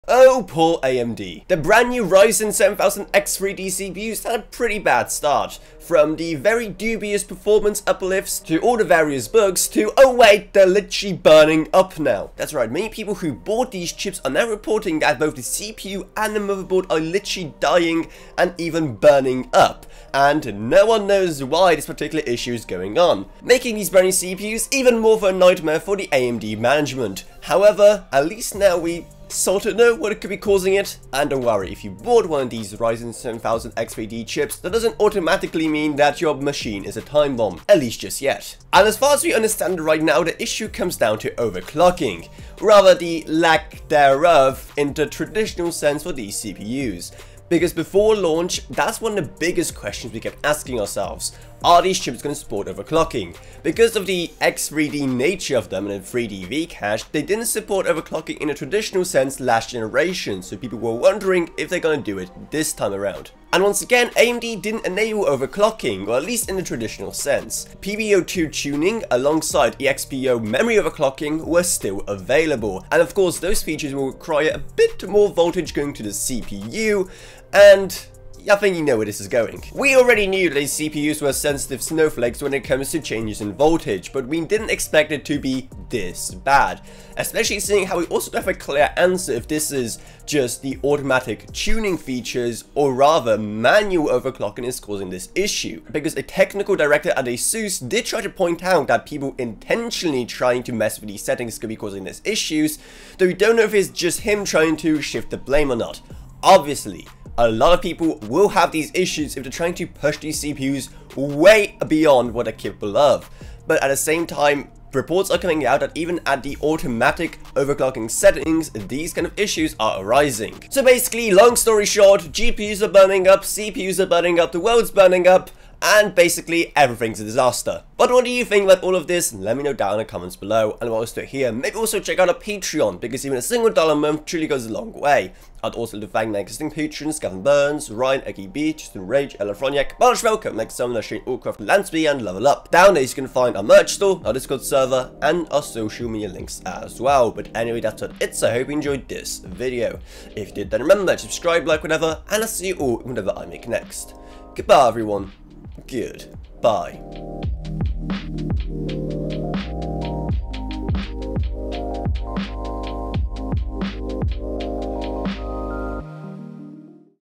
Ryzen 7000X3D Chips are BURNING UP?!
AMD Ryzen 7000X3D CPUs have already had a rough start, however no one could've predicted how bad it would go. While Ryzen 7000X3D overclocking has always been a difficult issue for AMD to address due to the 3D V-Cache, it looks even PBO2 and EXPO overclocking is making Ryzen 7000X3D CPUs burn up!